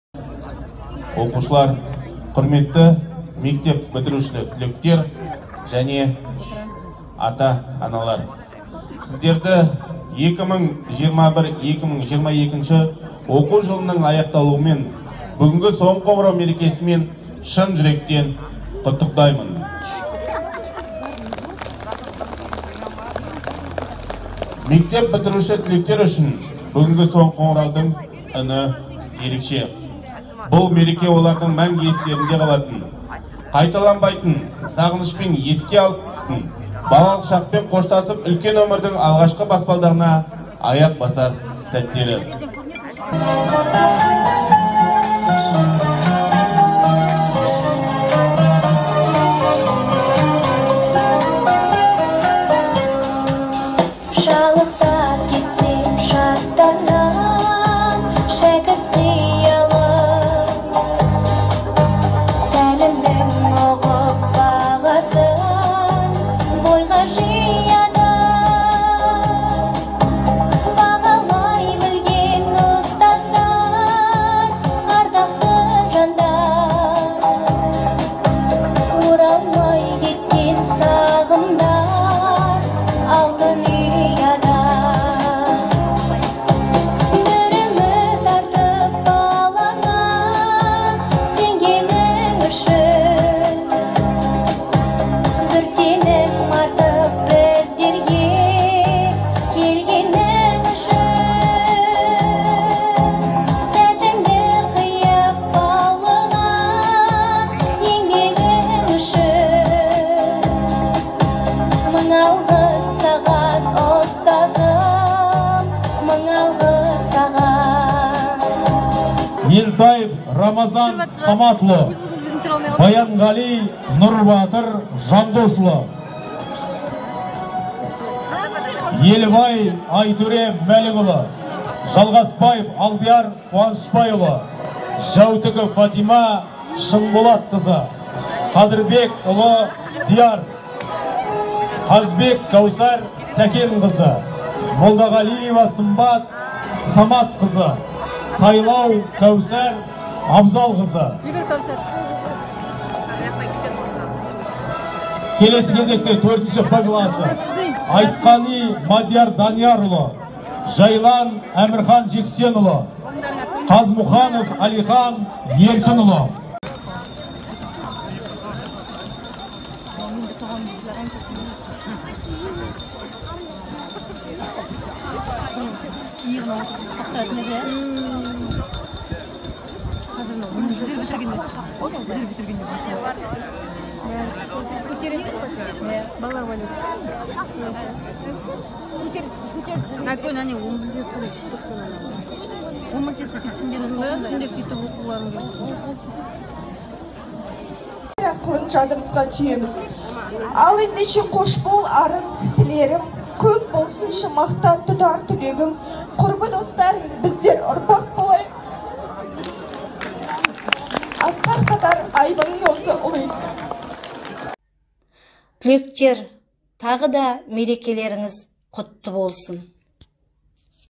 Сыңғырла, соңғы қоңырау! Мұхтар Арын атындағы Қарғалы қазақ орта мектебі.
Сайлау Каусар Абзалқызы - бастауыш мектеп үздігі!